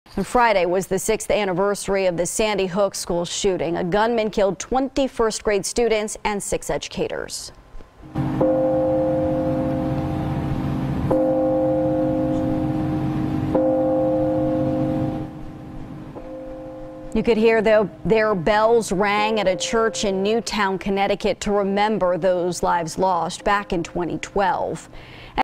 Bells Chime To Remember Sandy Hook Victims In Newtown
A gunman killed 20 first-graders and six educators.